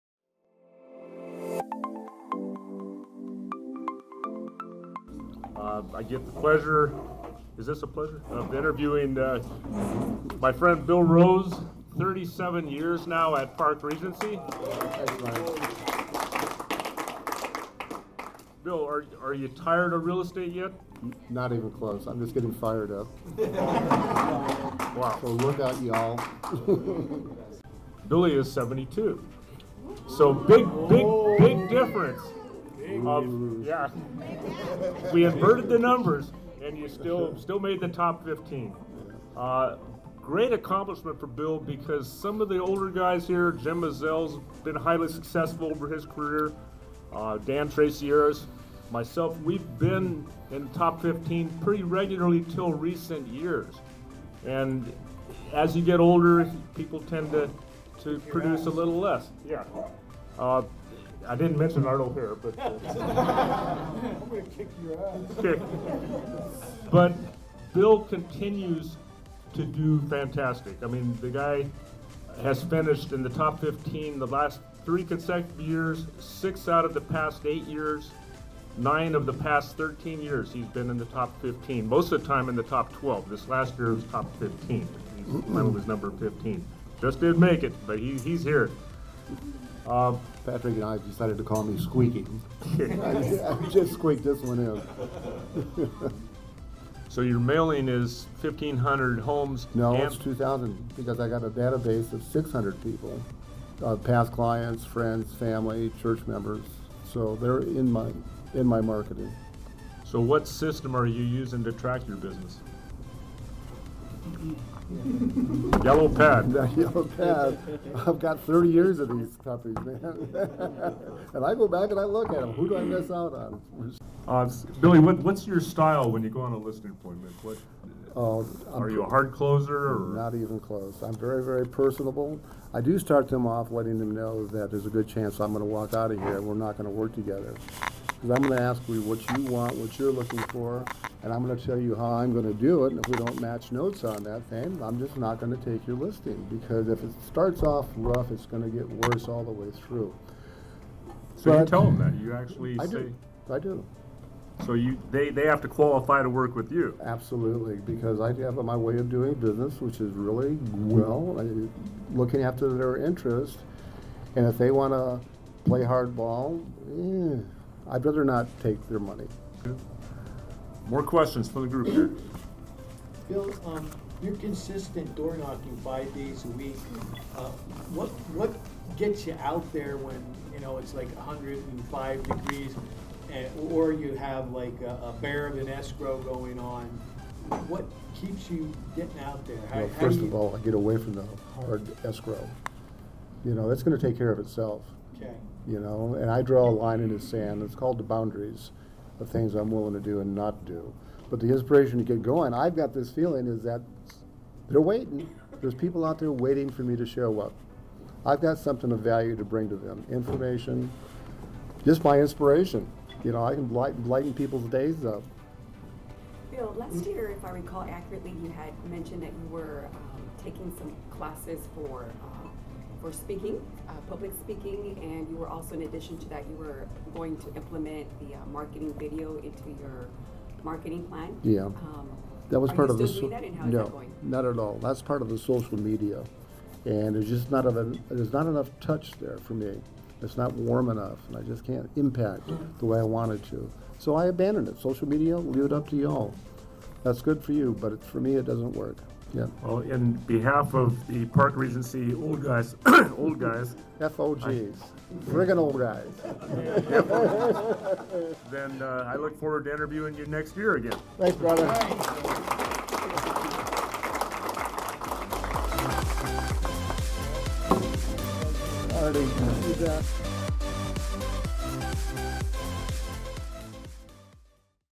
Bill's short version final
Park Regency Top Producer Bill Rose shares his keys to success with other agents.